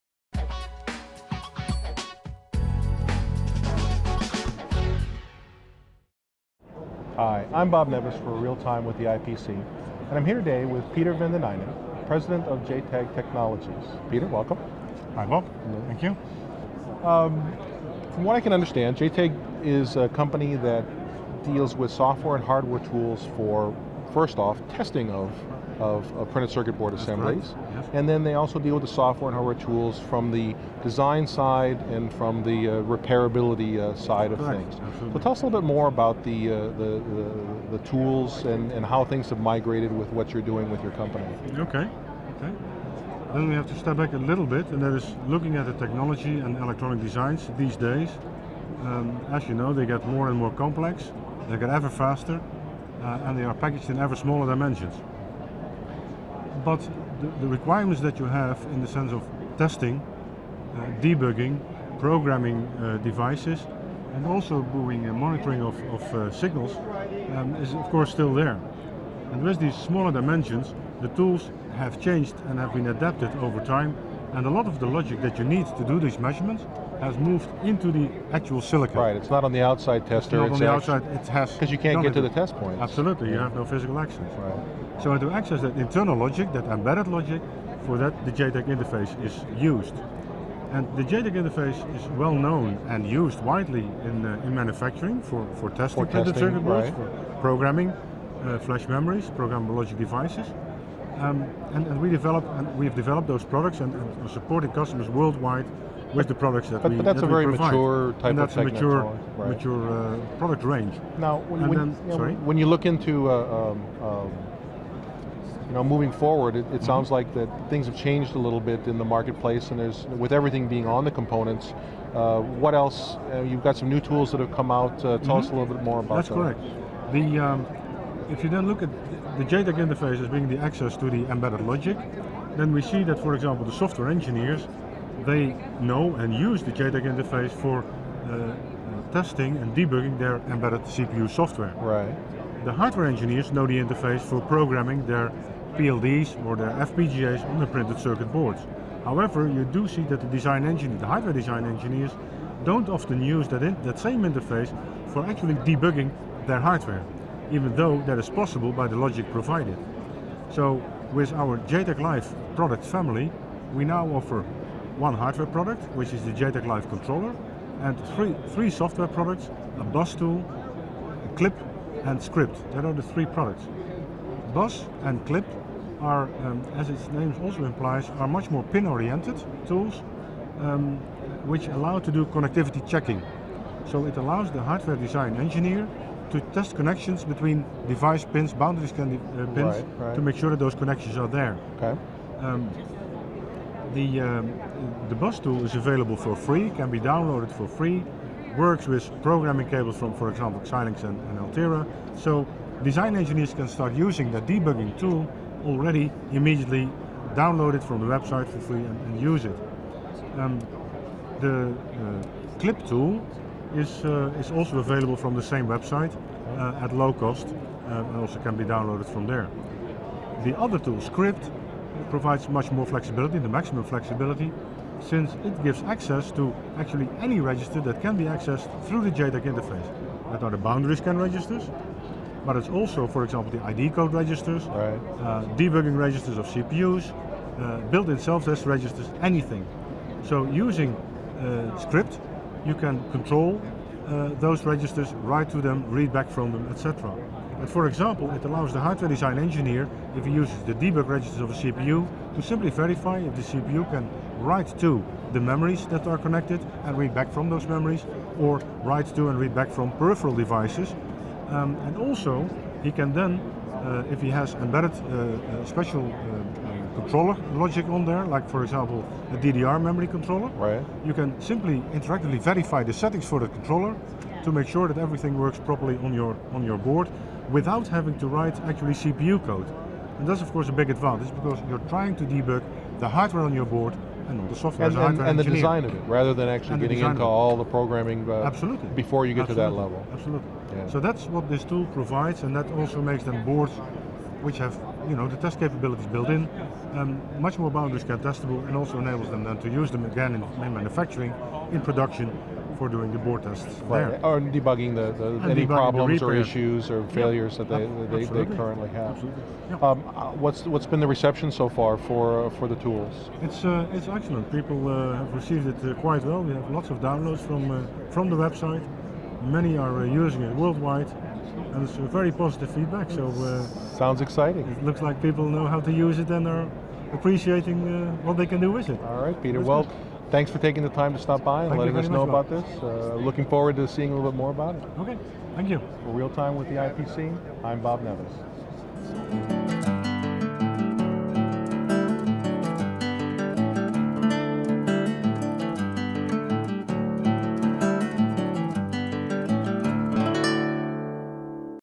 JTAG Technologies' President Peter van den Eijnden discusses boundary-scan for hardware design debug
JTAG Technologies CEO & President Peter van den Eijnden interview at Apex 2011 exhibition and conference. Peter discusses technology and JTAG Live products for hardware debug.